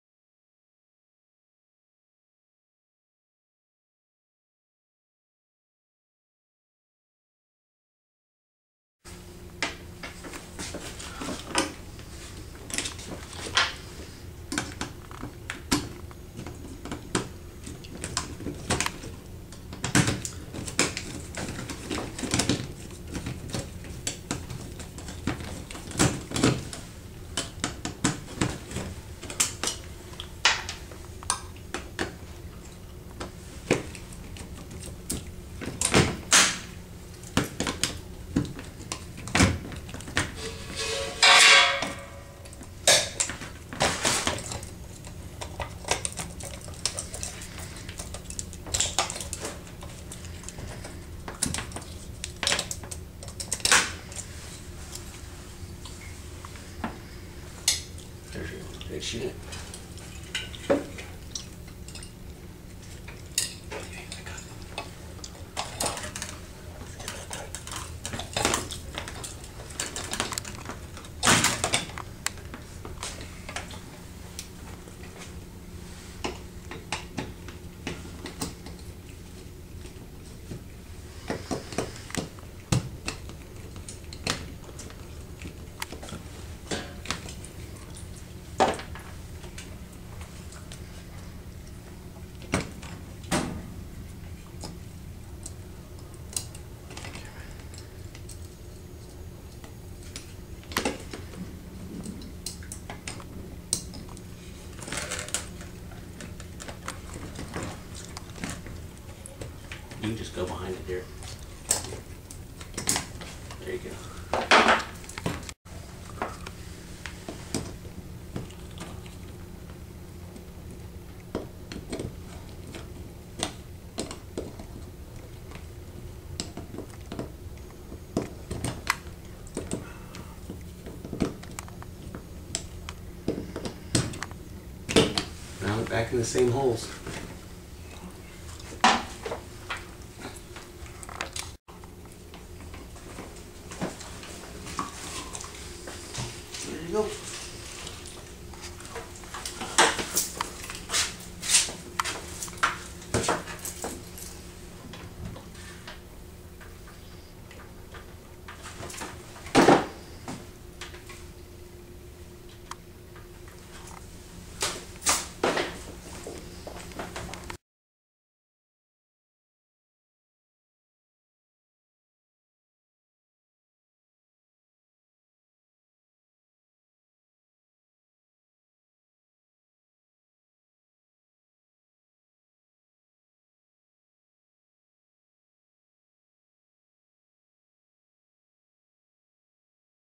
Marriott Temperature Control Change
eCube helps Marriott Plaza San Antonio, Texas
Certified Energy International helps Reduce Energy consumption in the Commercial Refrigeration Equipment at the Marriott Plaza in San Antonio, Texas using eCube. The eCube is one of the latest advances in Energy Conservation Technology.